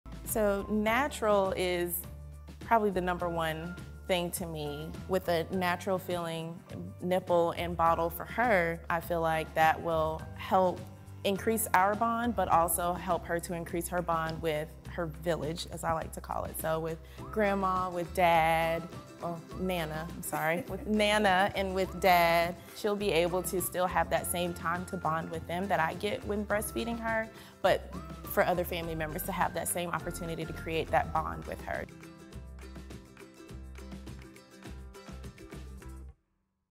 how can bottle choice help baby bond with their village?
What makes the mōmi bottle a truly "natural" choice for your baby? And how can the right bottle promote family bonding? In this video, Nicole shares how mōmi creates opportunity for her baby to bond with the whole family.

Since mōmi mimics the natural breastfeeding experience, Nicole felt confident that her little one could transition between breast and bottle.

If you’re looking for a bottle that supports a natural feeding experience and helps your baby connect with the people who love them, this video is a must-watch!